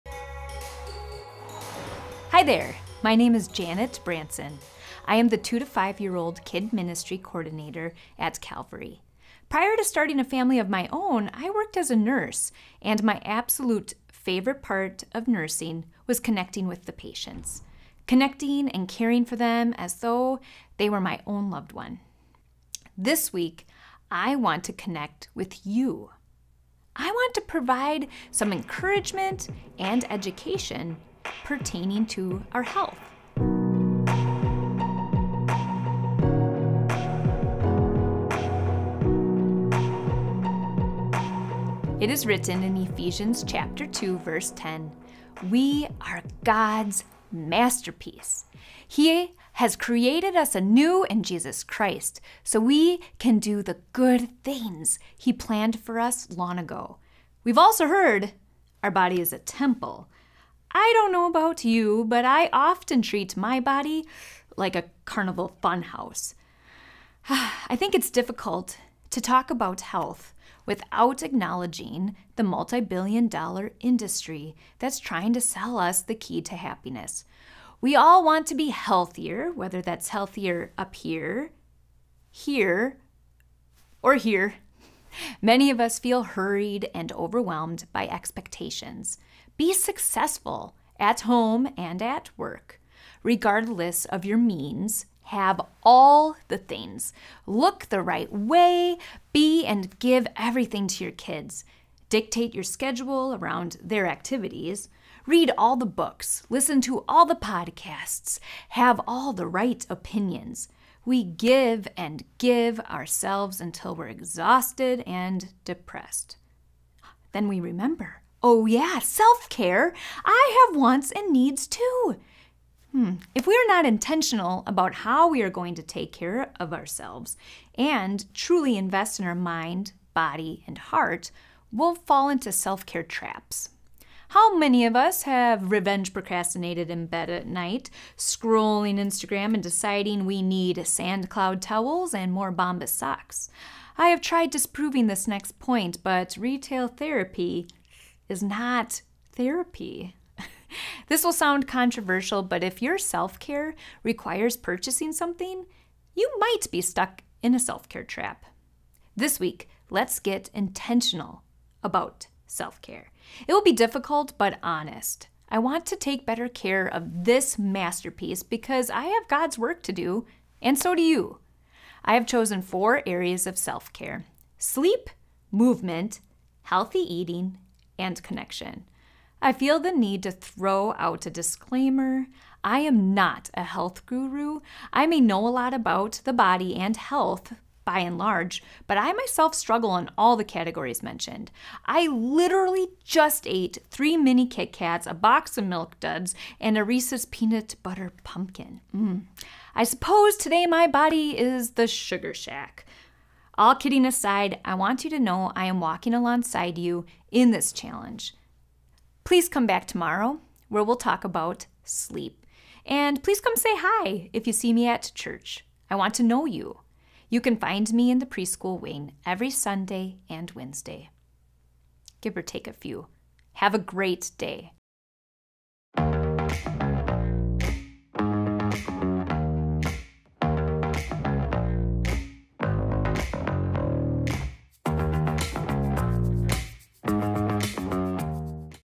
Universal Self-care // Janet Branson
On today's Daily Dose: Universal Self-care // Janet Branson

Caring for God's Masterpiece- YOU. In a world where self-improvement and self-care sells, how do we decide what's best for our bodies, mind and heart? Janet, nurse and Jesus and Me Coordinator for the 2-5 year olds at Calvary, will lead us through a medical and spiritual approach to self-care.

To financially support the ministry of Calvary and invest into the lives of others

Calvary Church is located in Alexandria, Minnesota.